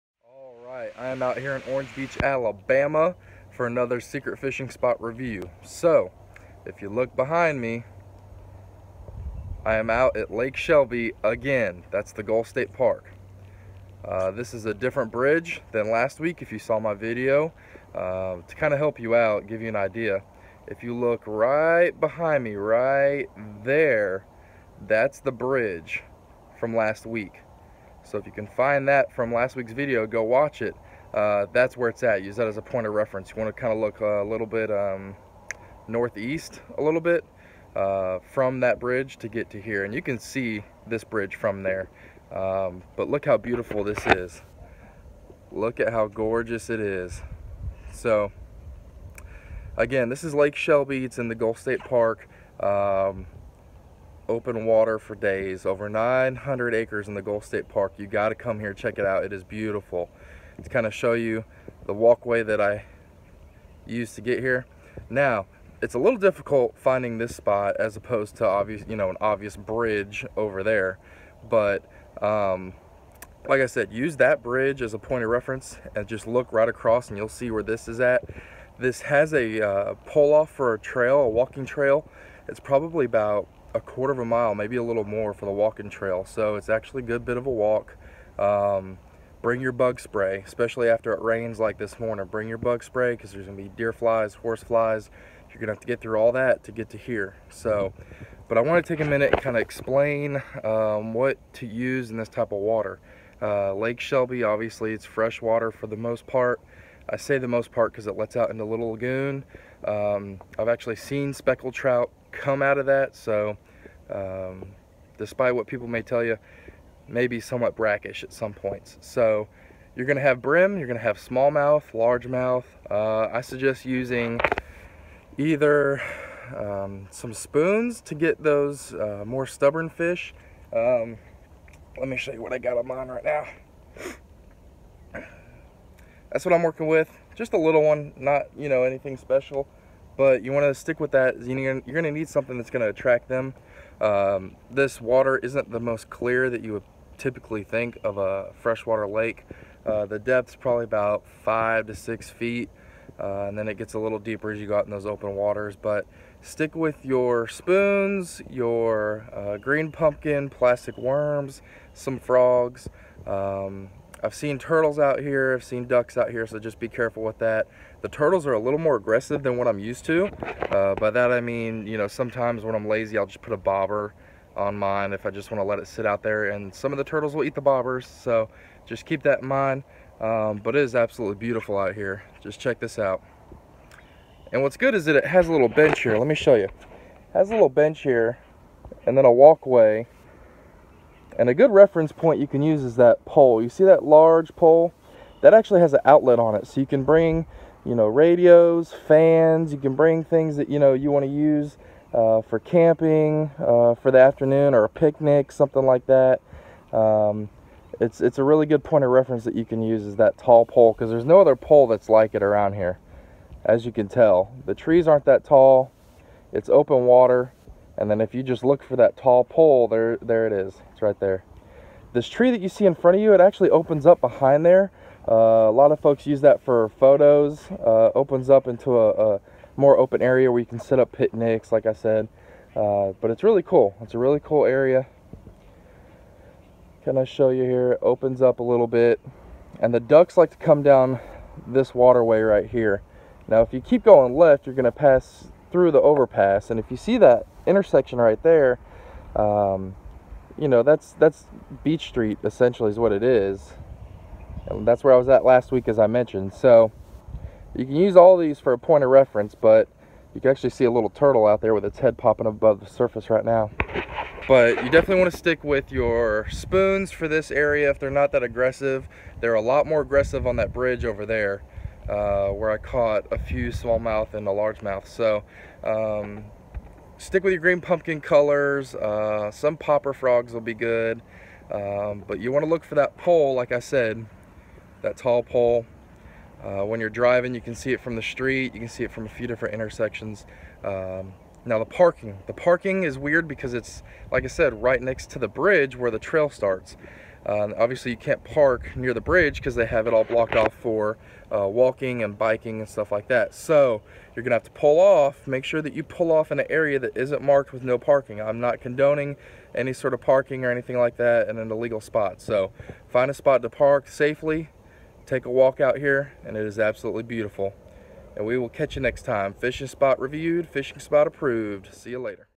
Secret Fishing Spot! Orange Beach, AL (Gulf State Park - Gopher Tortoise Trail)
Today, I review another secret fishing spot in the Gulf State Park.